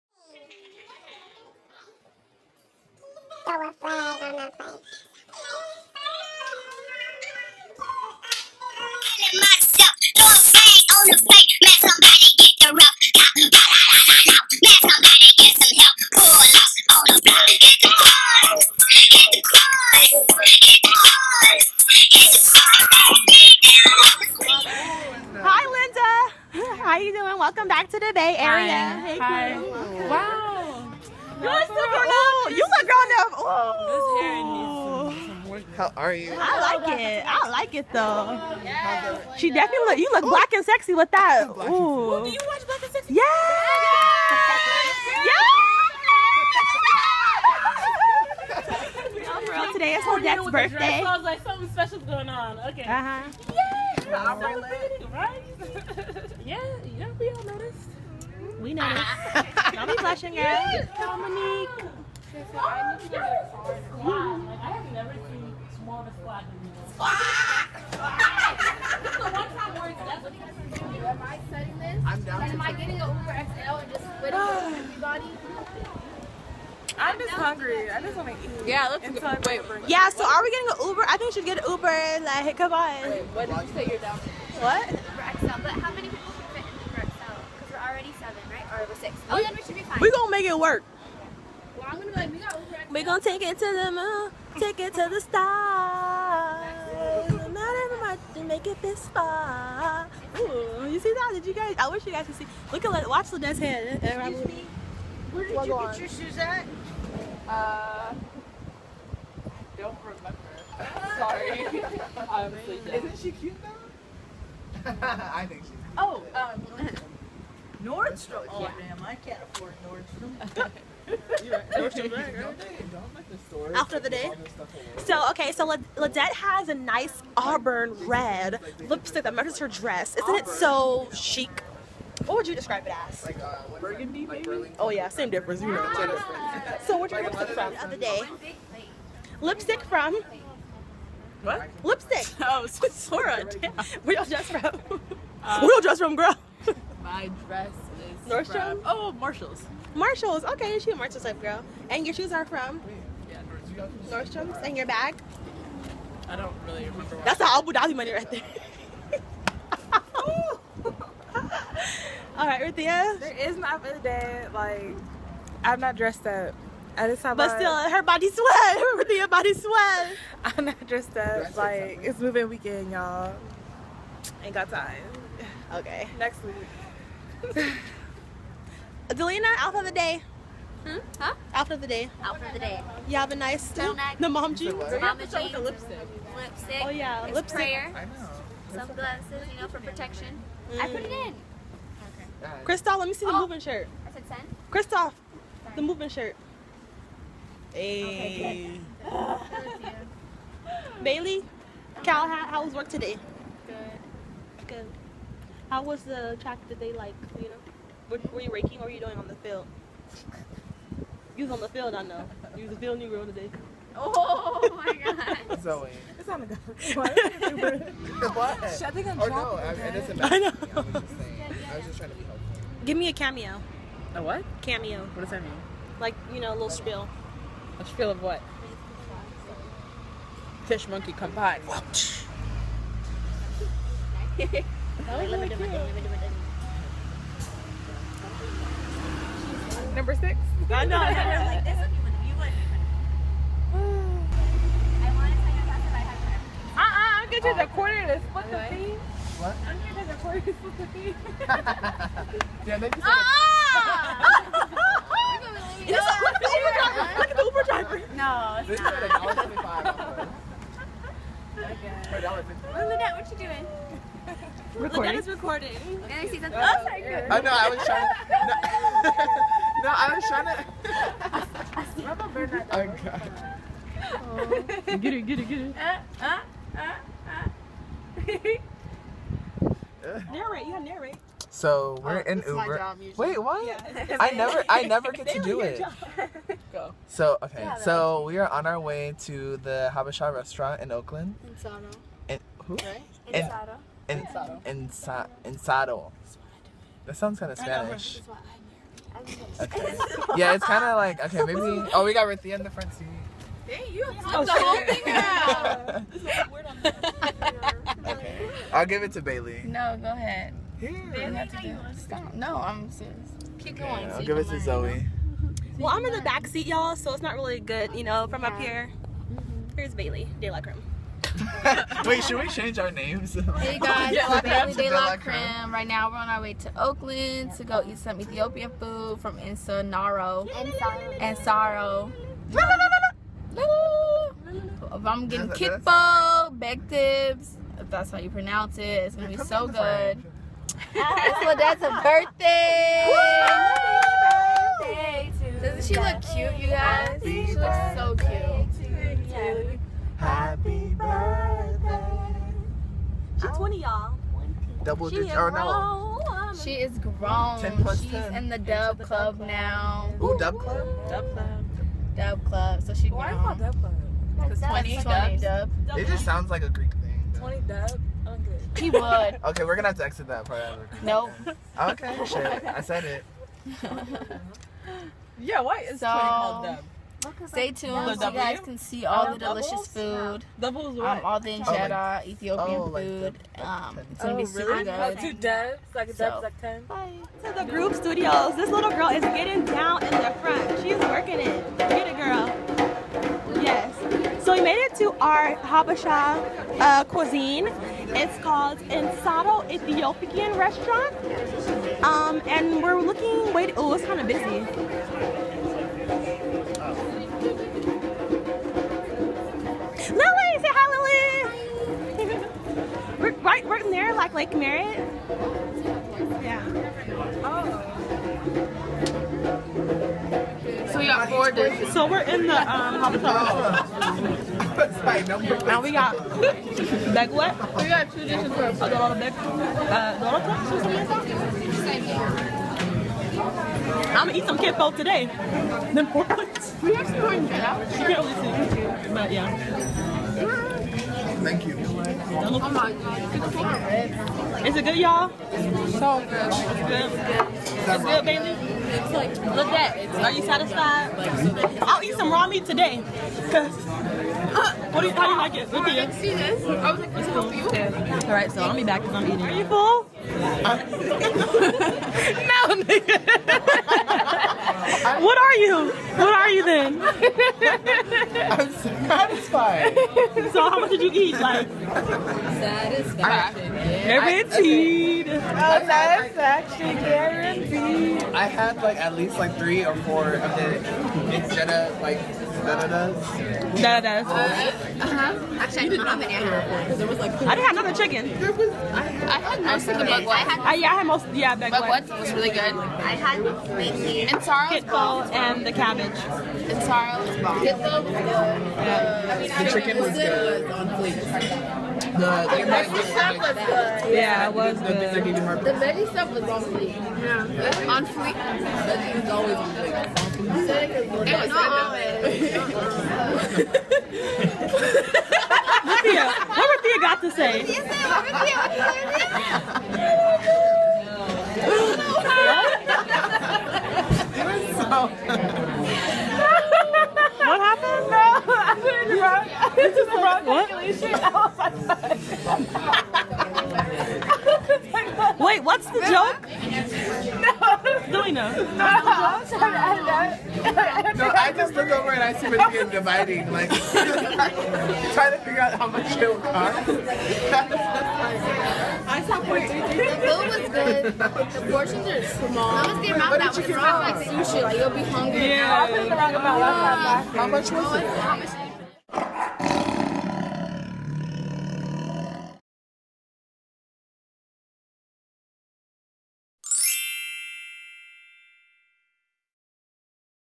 zoeydelacreme Vlogs- Ledet's Birthday
20 years young. One of my favorite east African girls!

We went to Insaro Restaurant in Oakland, CA